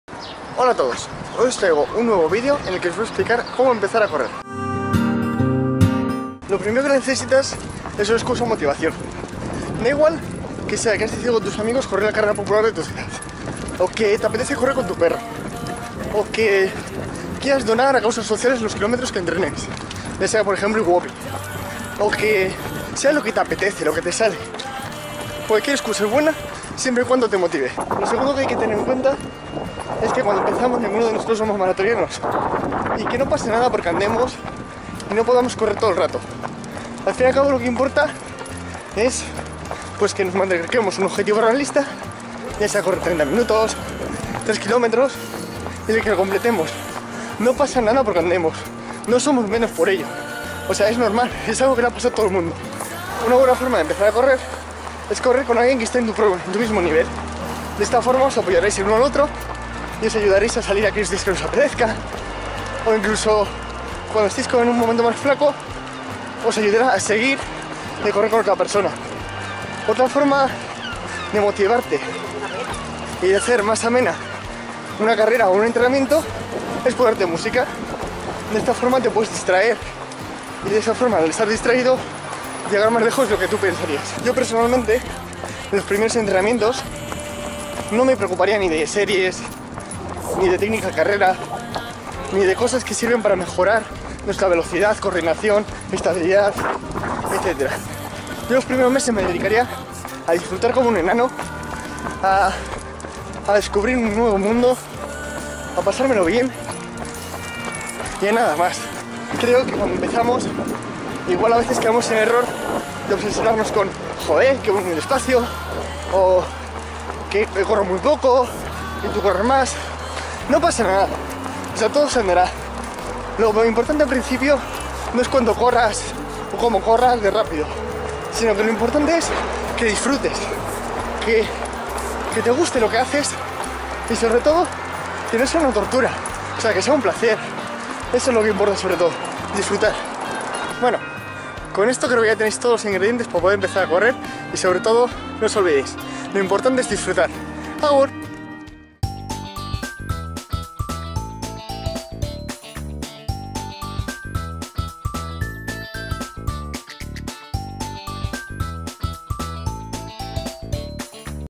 Cómo empezar a correr! Trucos, motivaciones y consejos
En este vídeo os cuento trucos, consejos y recomendaciones para que empecéis a correr y sobre todo disfrutéis.

Lo importante es disfrutar!!!

Podéis donar vuestros kilómetros en las distintas causas solidarias de iwopi.

Por cierto si estáis pensando en viajar os dejo un código de 25€ de descuento para vuestra próxima reverva en airbnb